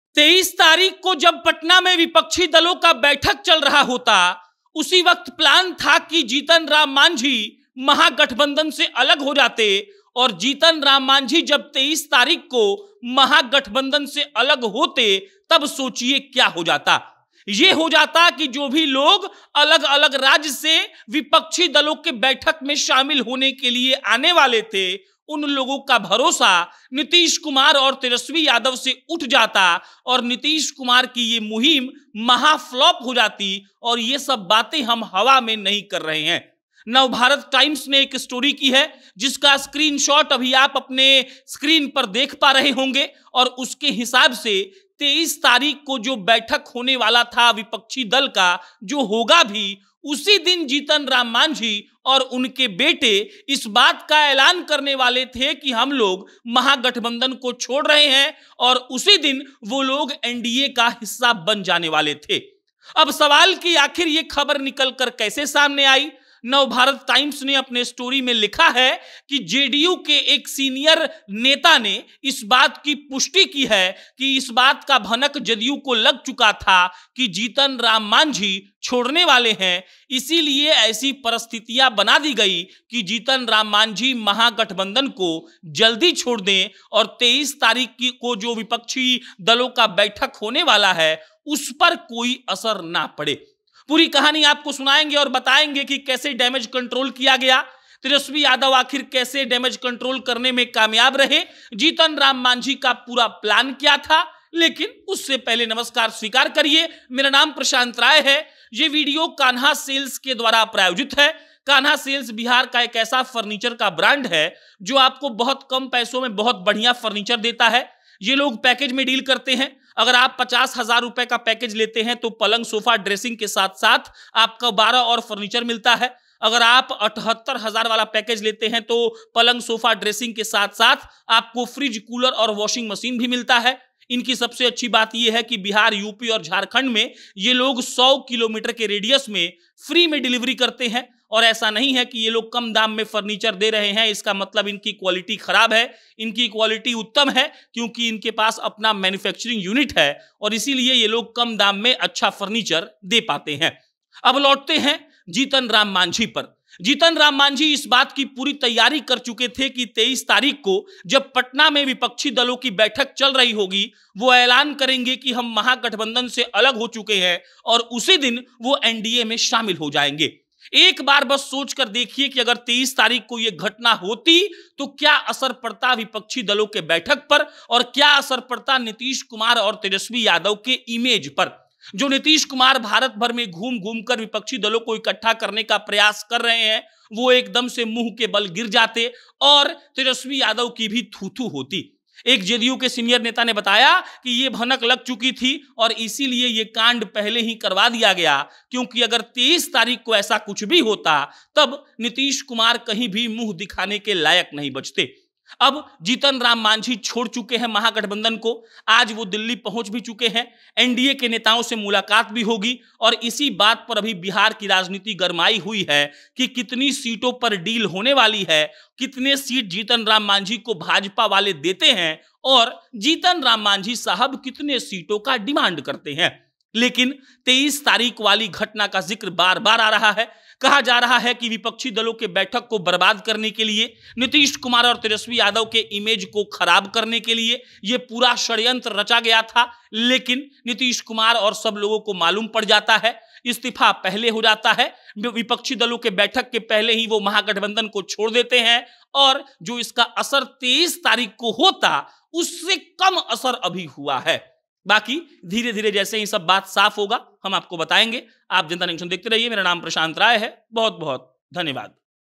Tejashwi और Nitish की फ़ज़ीहत करने वाले थे Manjhi, ऐन मौक़े पर डैमेज कंट्रोल किया गया, सब जानिए!
जिस दिन विपक्षी दलों की बैठक होने वाली थी उसी दिन महागठबंधन छोड़ने वाले थे जीतन राम माँझी, नीतीश कुमार और तेजस्वी यादव को बड़ा झटका देते लेकिन उससे पहले से जदयू वालों ने खेल कर दिया! पूरी कहानी जान लीजिए!